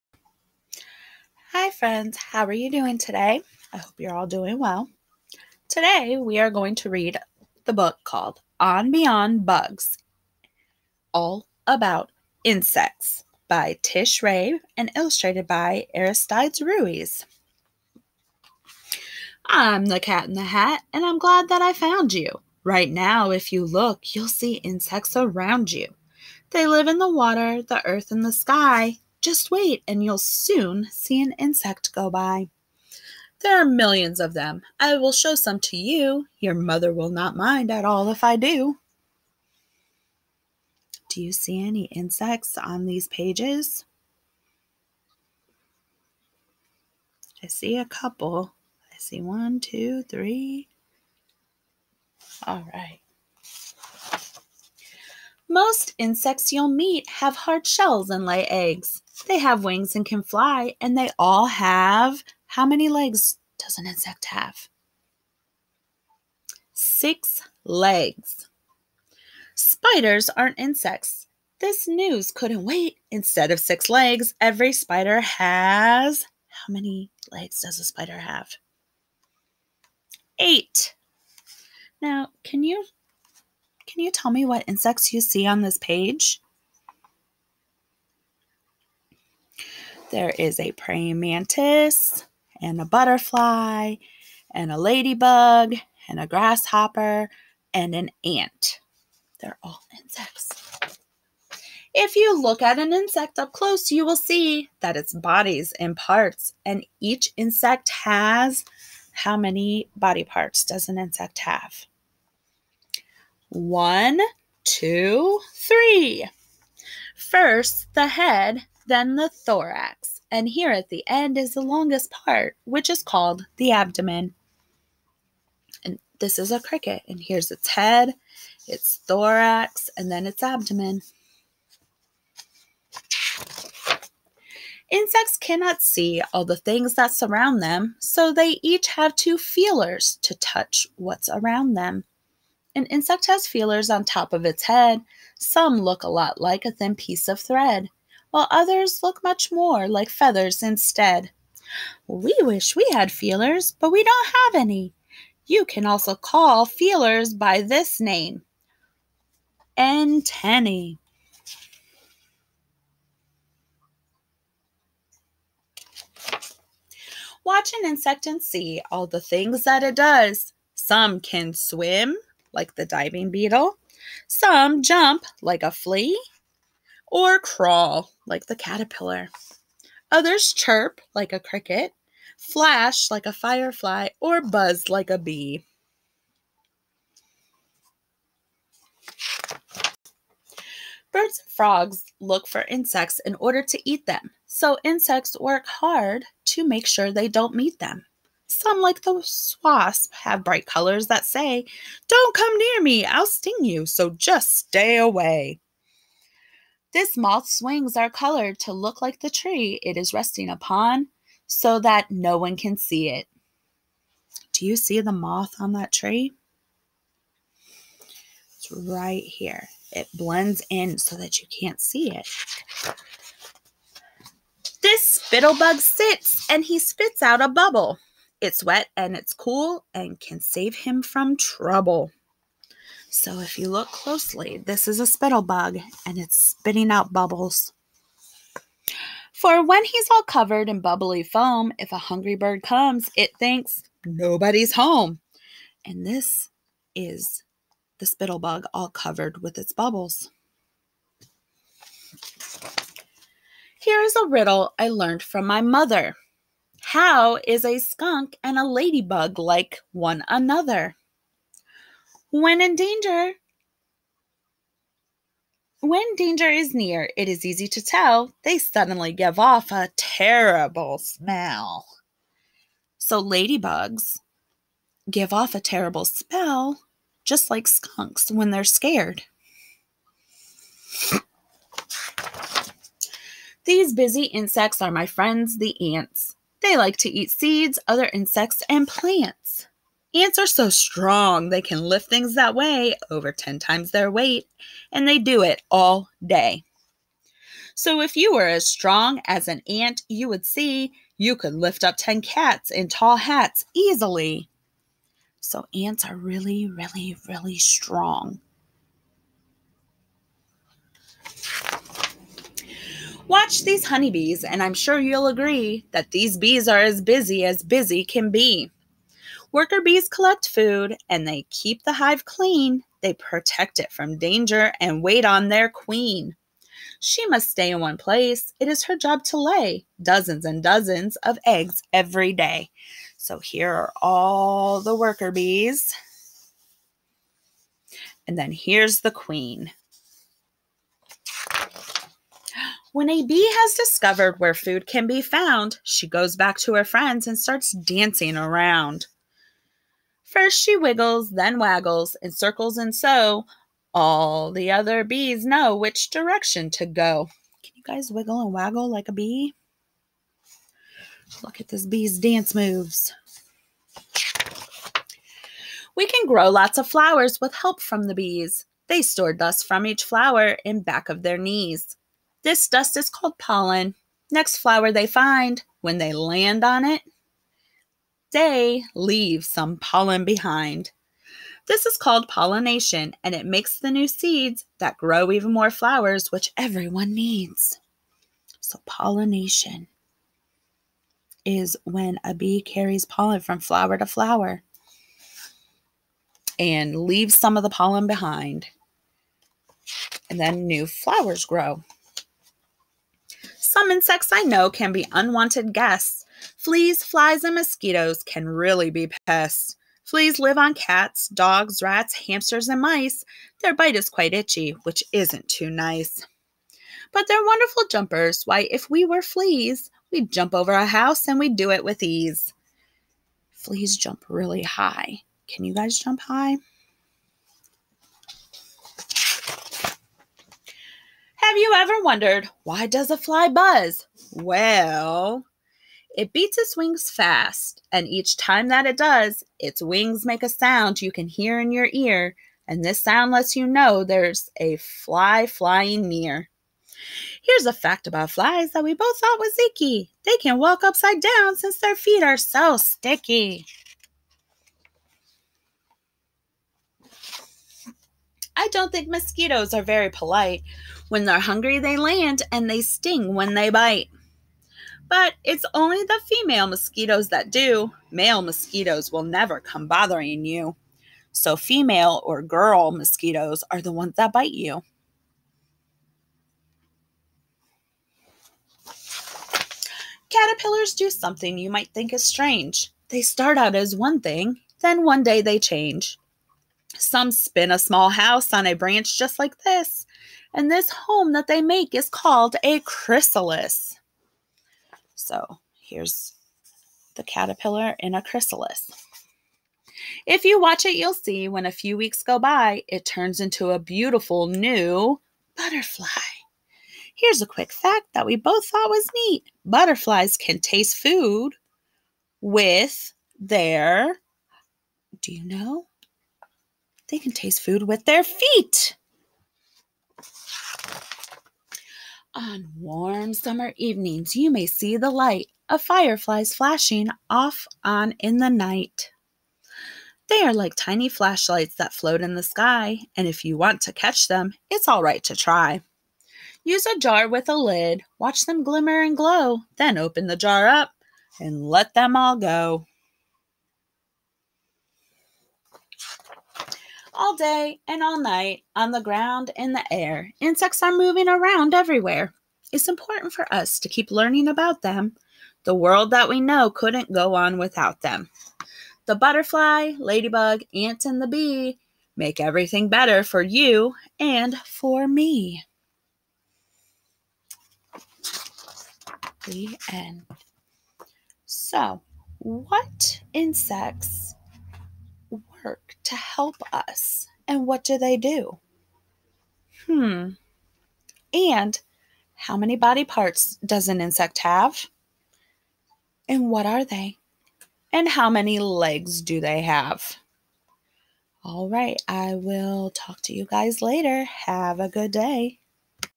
"On Beyond Bugs! All About Insects" by Tish Rabe, Illustrated by Aristades Ruiz
Talk to your child/children about different insects. We were discussing the parts of an insect at school (Head, thorax, abdomen, eyes, mouth, antennae, and 6 legs). Ask if they can teach you the insect song. Discuss some of the insects and what they do.
If you have any documentation of your interactions throughout this story, please send it our way!
We appreciate you! Have a great day!